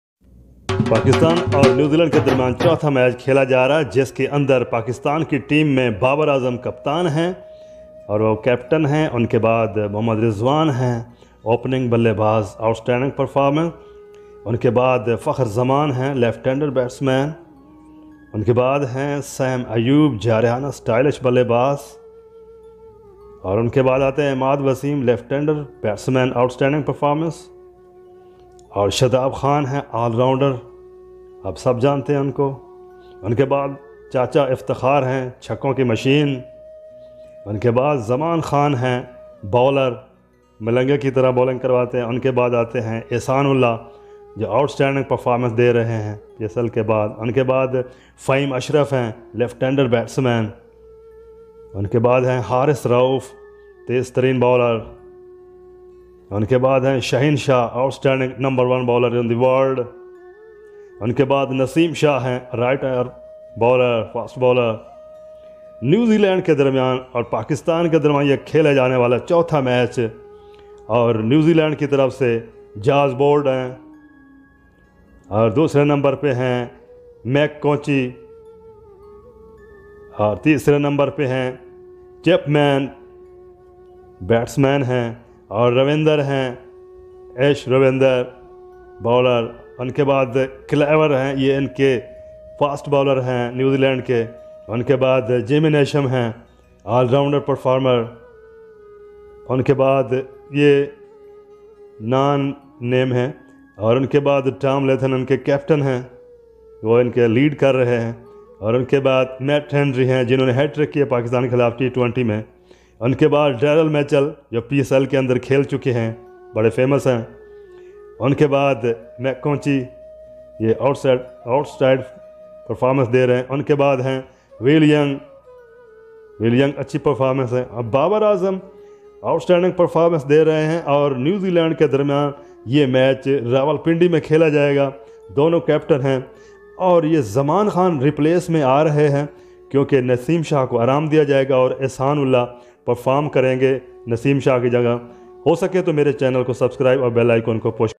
New Zealand vs. pakistan 4th matchT20 2023 in pakistan [pak vs. New Zealand ]
pakistan vs new Zealand team announced  for 4th match 2023 t20 plying in Rawalpindi stadium. 20 April 2023 time to 8:30 pm.